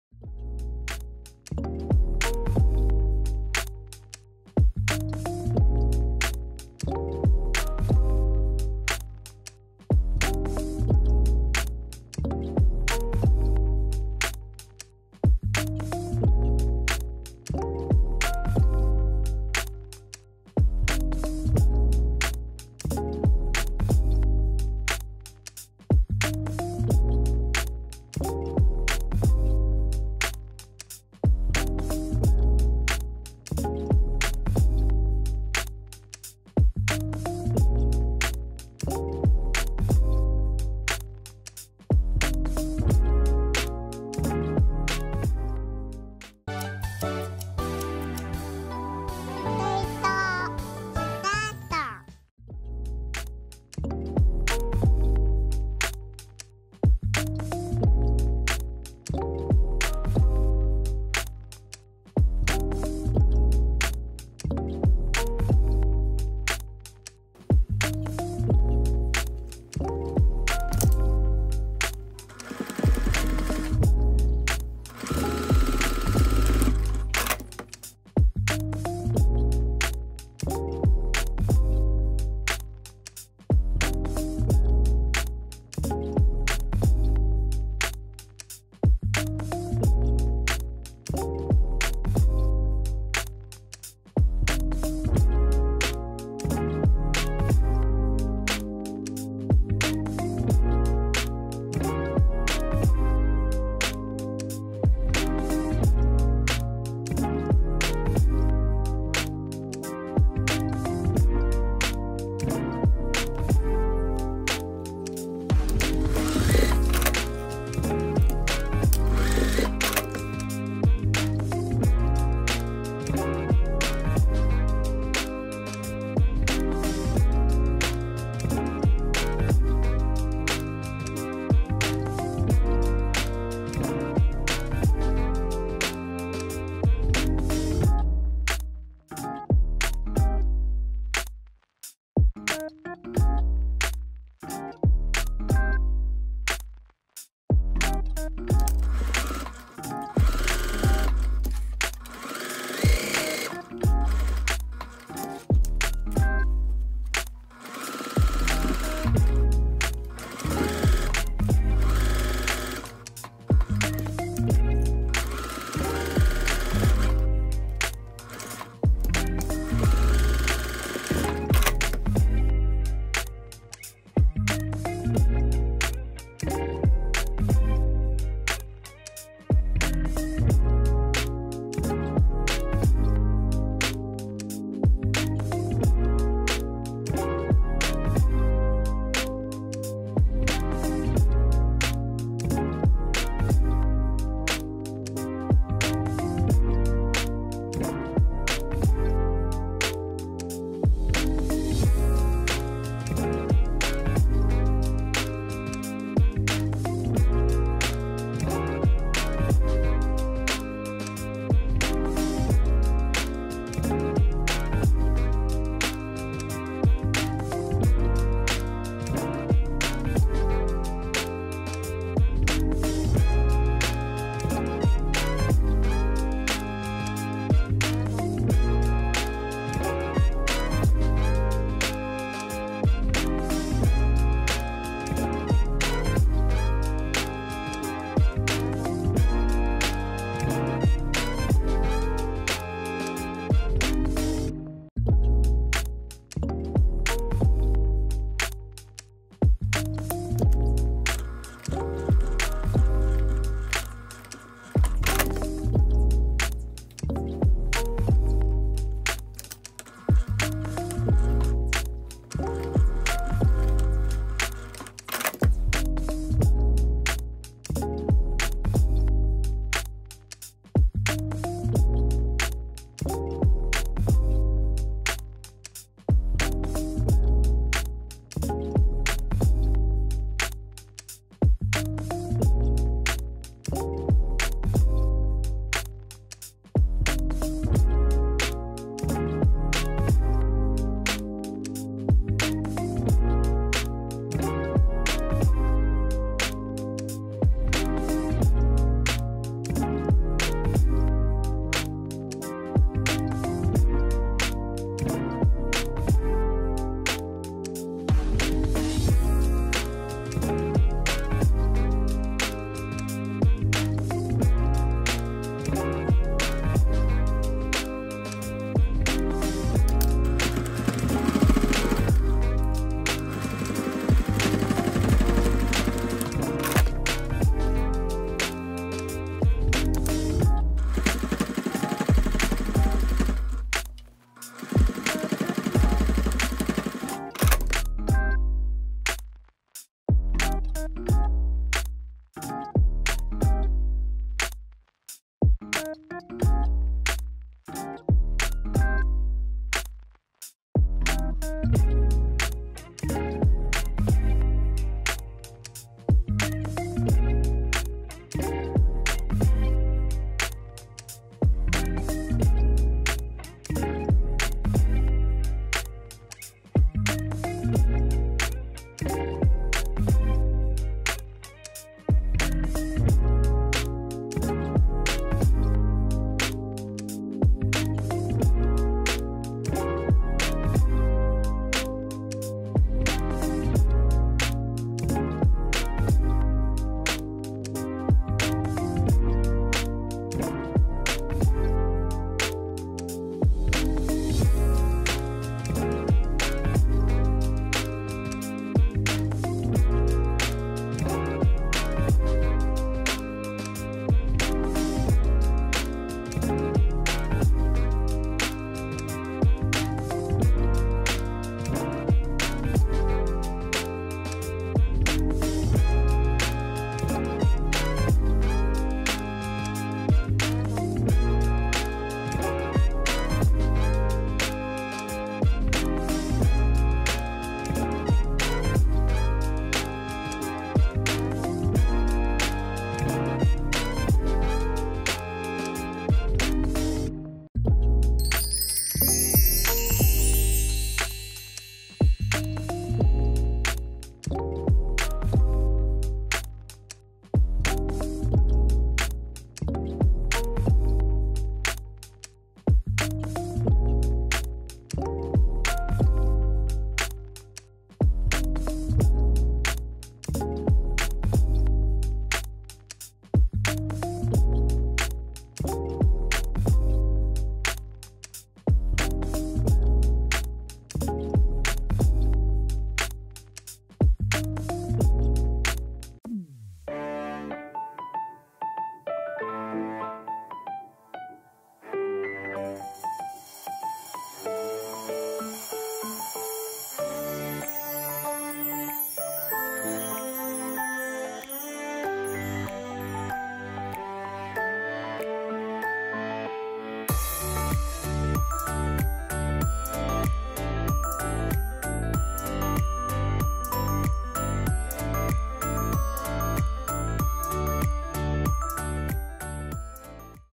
スリムコインケース♡携帯に良き❣️シンプルが良き👍いといとitoito
2枚から作る、簡単コインケース♡
キャッシュレス時代でも、やっぱり現金は必要な時も…そんな時に少しだけ持ち歩きたい方必見☺️🤲
カバンに付けたりできるので、小さくてもなくならないよね💕︎
百均でも合皮が購入出来たりするので、是非✨️チャレンジしてみてくださいね🎶
もちろん、普通の布でも作れます♡♡

コメントやリクエストもお待ちしてます🎶

動画を見てくださってありがとうございます🙇‍♀️作った作品をSNSでご紹介される際に いといとitoito と付けてくださると大変喜びます🥰よろしくお願いします🙇‍♀️